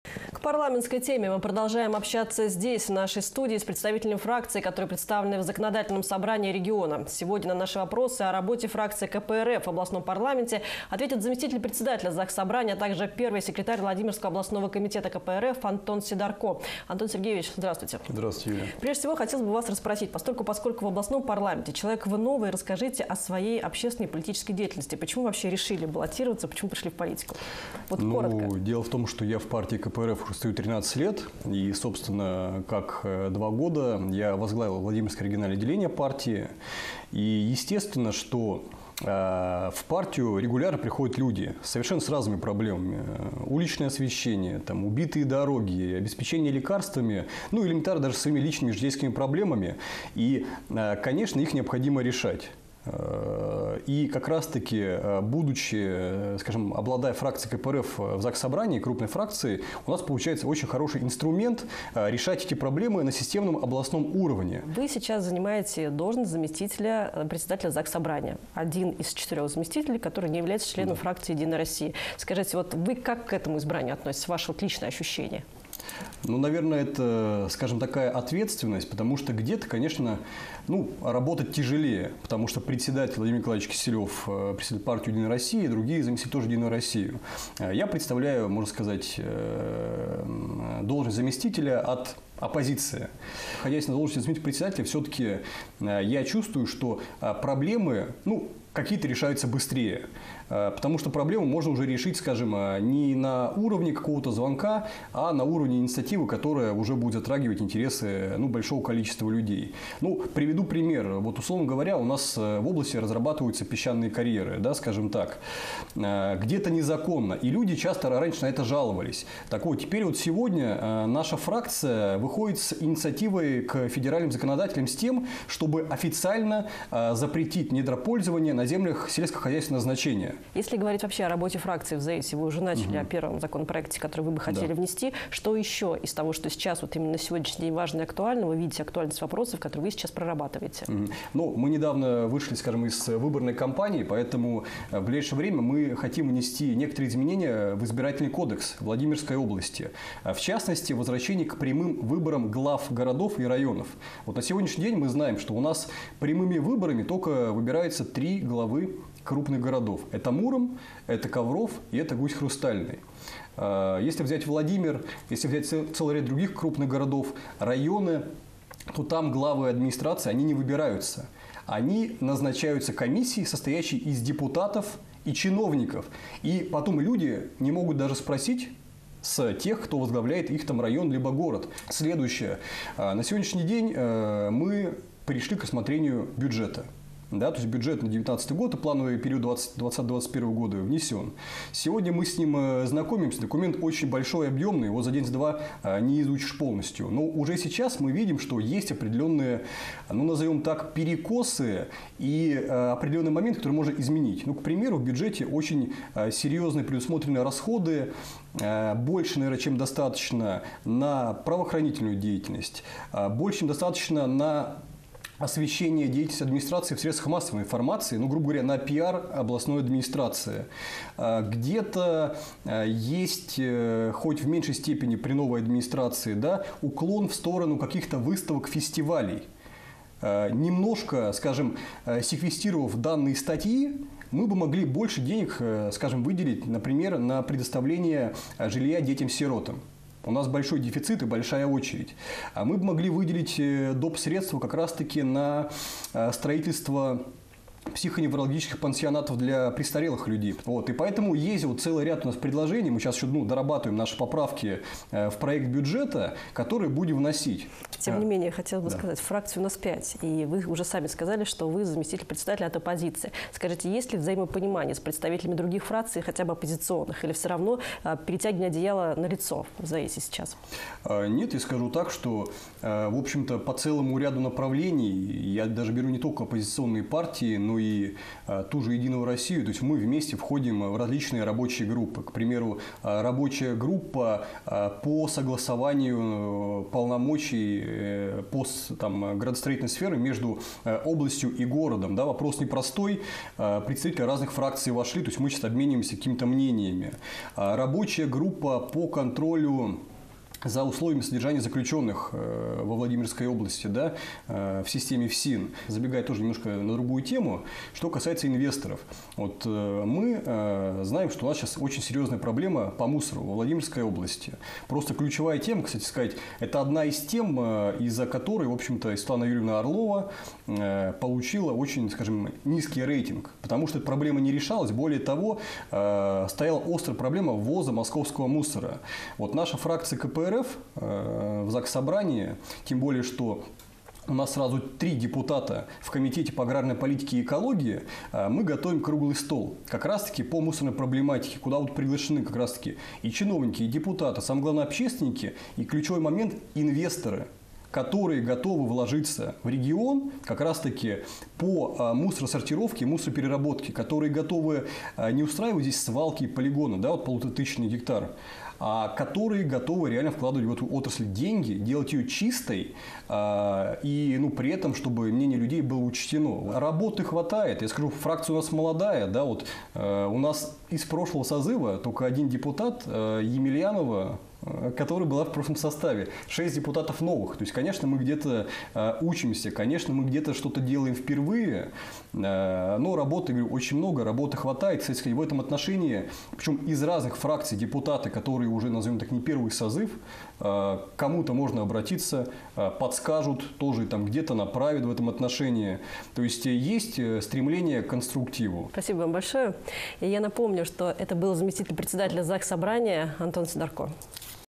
Альтернативный взгляд на бюджет, выборы глав и мусорный вопрос
К парламентской теме. Мы продолжаем общаться здесь, в нашей студии, с представителями фракций, которые представлены в Законодательном собрании региона. Сегодня на наши вопросы о работе фракции КПРФ в областном парламенте ответил заместитель председателя ЗакСобрания, а также первый секретарь Владимирского областного комитета КПРФ Антон Сидорко.